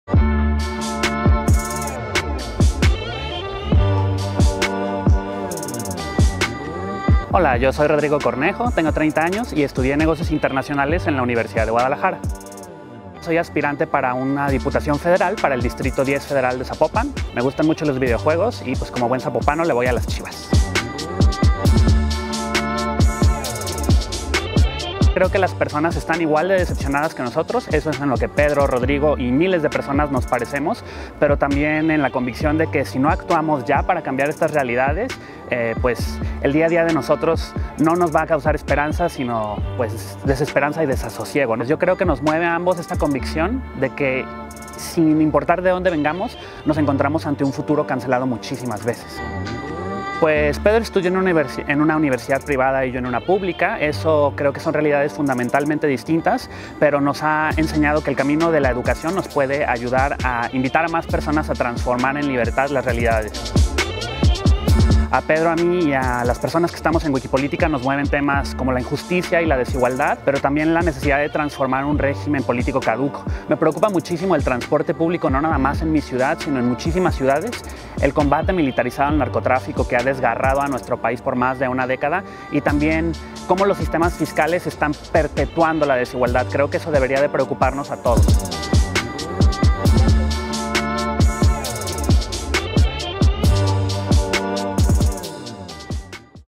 Rodrigo Cornejo: haciendo política estilo Kumamoto
Él es Rodrigo Cornejo, integrante de Wikipolítica Jalisco, aspirante independiente y uno de los jóvenes que está haciendo política al estilo de Pedro Kumamoto